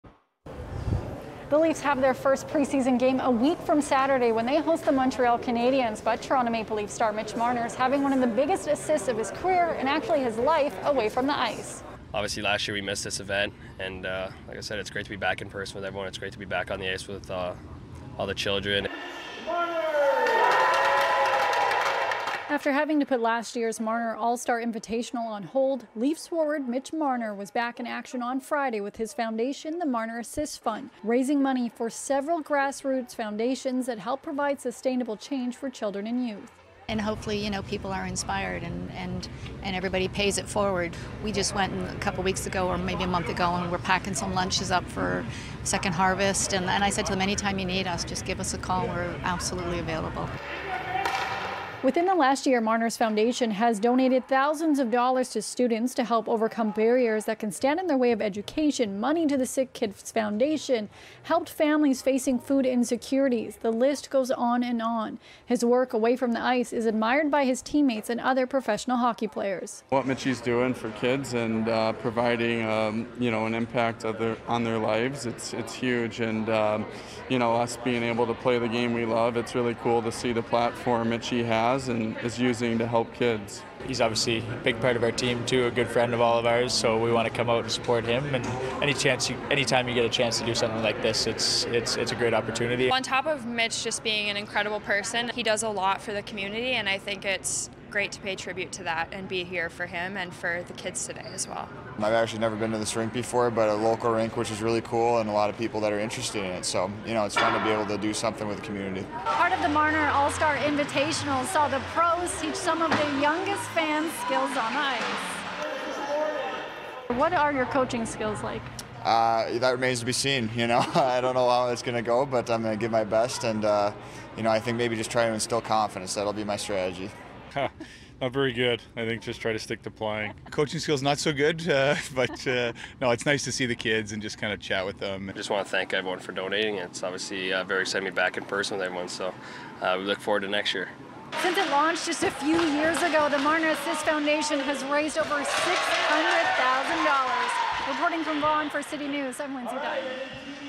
Leafs Marner helping change lives off the ice
Toronto Maple Leafs pre-season is less than a week away, but before the puck drops on a new season Leafs Forward Mitch Marner is continuing to make a difference off the ice. Lindsay Dunn spoke to Marner about the Marner All-Star Invitational.